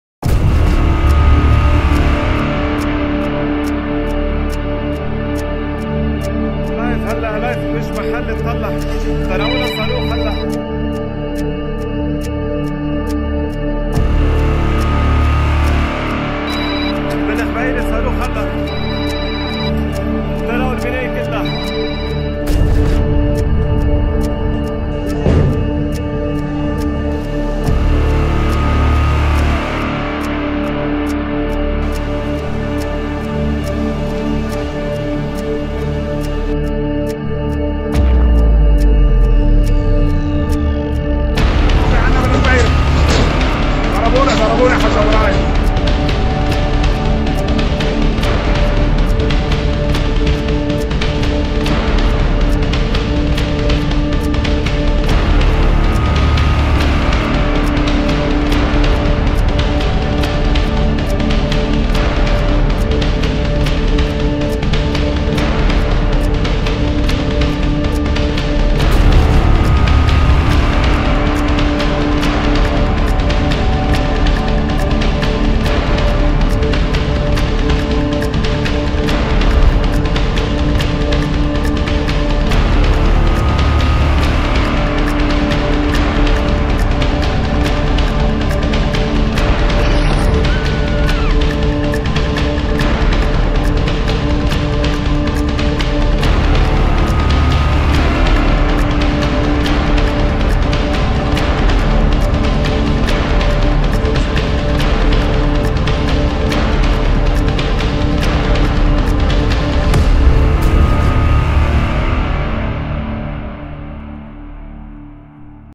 حزب الله يقر بمقتل قائد منظومة الصواريخ بضربة إسرائيلية
حزب_الله يقر بمقتل قائد منظومة الصواريخ إبراهيم_القبيسي بضربة إسرائيلية على الضاحية الجنوبية
لمعرفة أحدث المستجدات.. عبر البث المباشر من هنا 👇
وبإمكانكم متابعة العربية على منصات التواصل